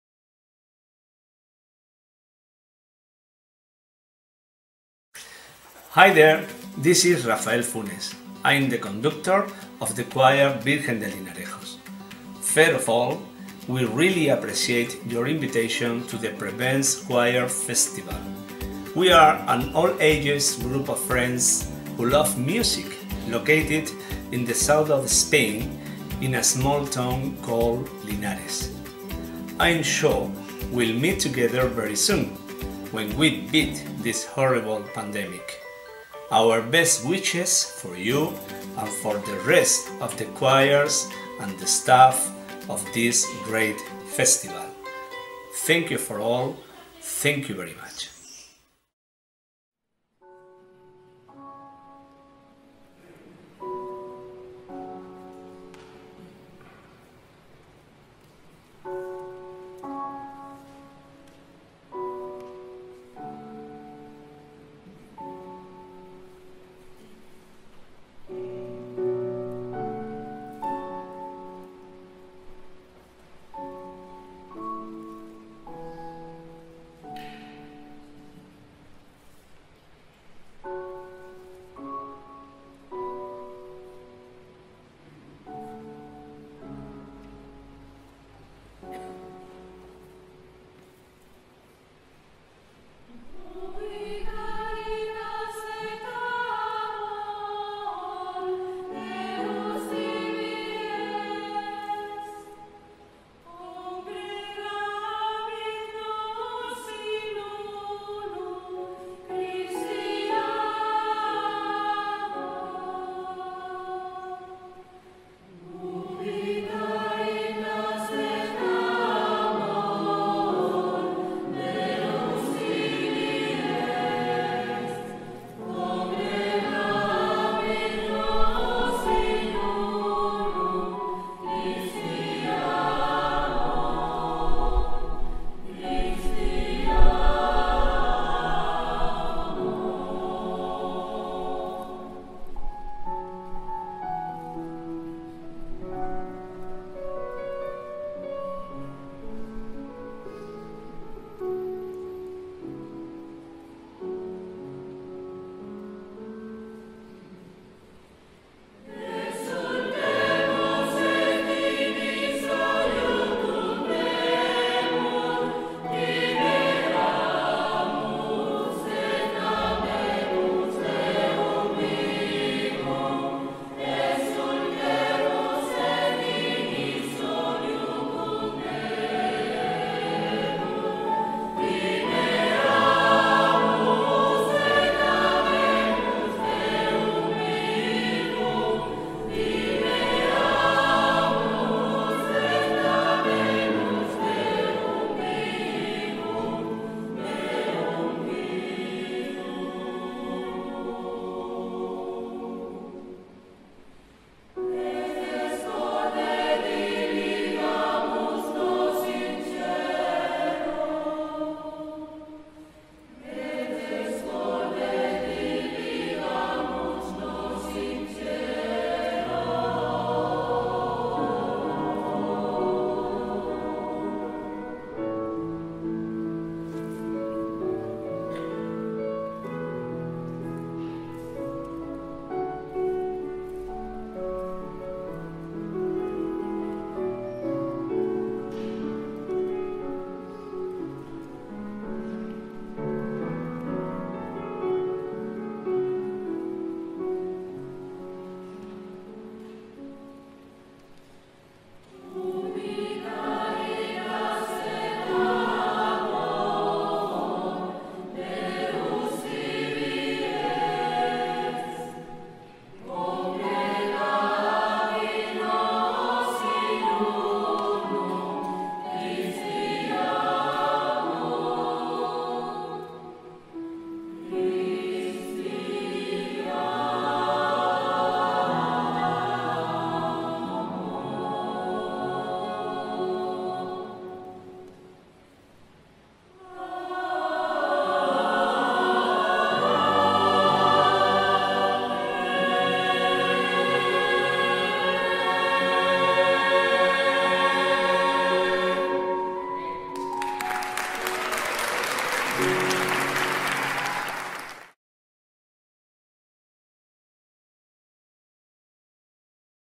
Virtual Festival 2020 - Coral “Virgen de Linarejos”
Virtual Choral Festival 2020
Organised by the choir "Armonia" of Preveza, in collaboration with the International Musical Festival of Thessaloniki

Coral “Virgen de Linarejos” , Linares, Spain

Conducted by: Rafael Funes Arjona